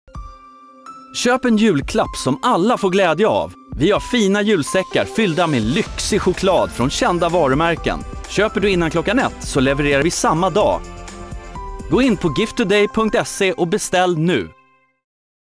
Julsäckar med choklad från GiftToday - Chokladogram ®
Julreklam från presentportalen GiftToday. GiftTodays ena licensierade tjänst Chokladogram ® erbjuder en julsäck fylld med choklad. Reklamfilmen går i tv4 under december 2008 och är 15 sekunder lång.